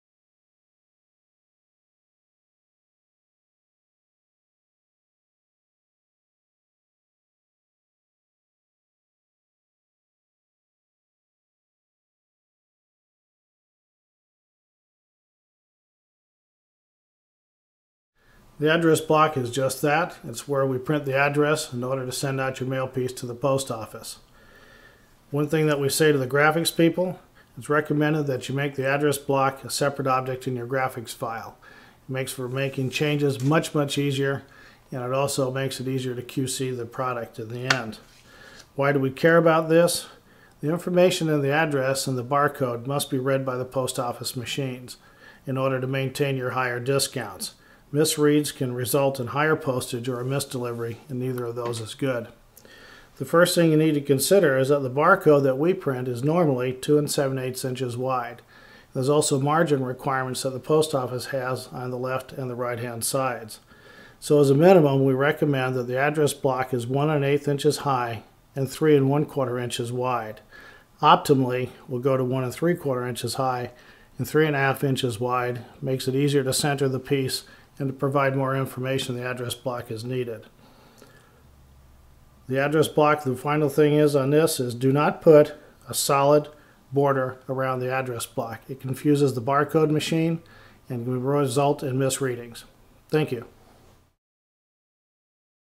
Address Block - Mail Piece Design Specs
Design requirements for the address block on any standard mail piece.

The template featured in this video does not represent the only way to layout your mail piece design. There are numerous other ways to satisfy USPS requirements. This template follows a conservative path to keep the designer in compliance with the design rules. Any mail house or designer is encouraged to verify their artwork with their local USPS mail piece design analyst.

Mailing Pros Inc.